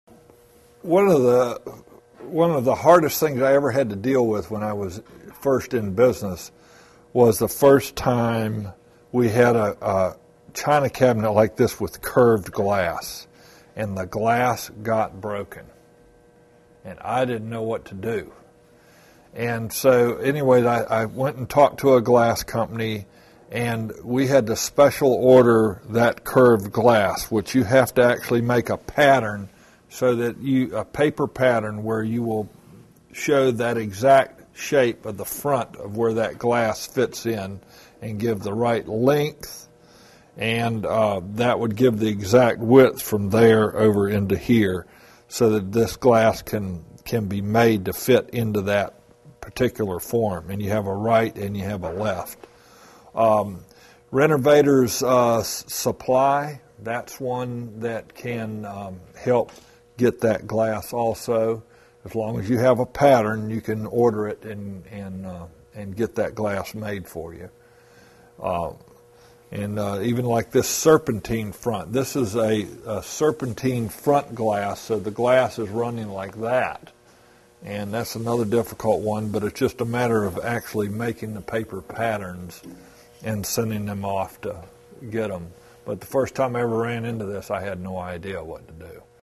Restoring Wood Furniture : Restoring Furniture with Curved Glass
Getting curved glass to fit furniture you are restoring can be very difficult. Get tips on matching curved glass to your furniture in this free video.

Expert: Ralph Jensen
Bio: Ralph has been restoring furniture for over thirty years in Wilmington NC.  He restores antiques, builds custom pieces, and does repairs on new furniture in his antique workshop.
Filmmaker: Reggie Hayes